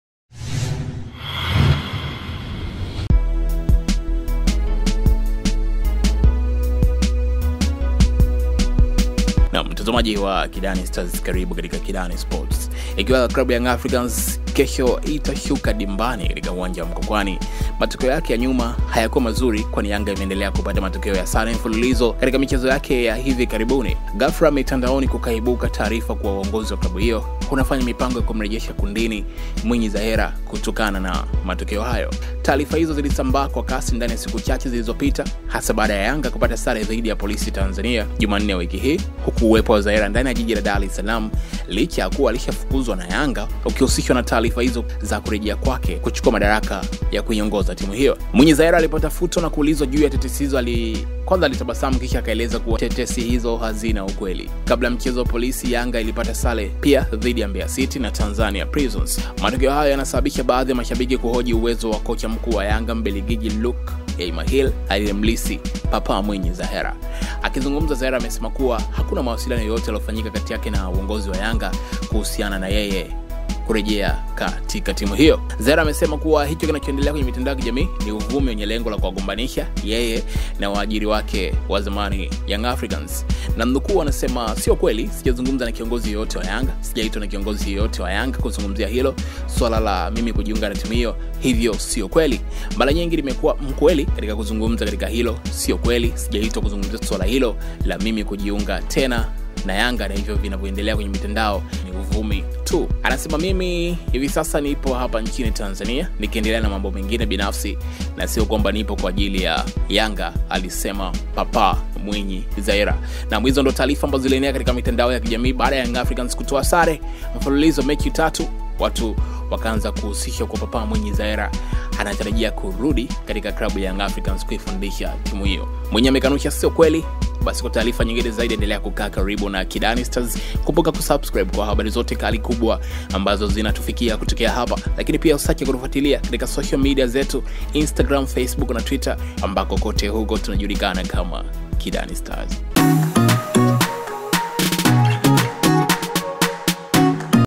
Tetesi za Kurudi YANGA Mwinyi Zahera Afunguka
YANGA imeendelea kupata matokeo ya sare mfululizo katika michezo yake ya hivi karibuni, ghafla mitandaoni kukaibuka taarifa kuwa uongozi wa klabu hiyo unafanya mipango ya kumrejesha kundini Mwinyi Zahera kutokana na matokeo hayo.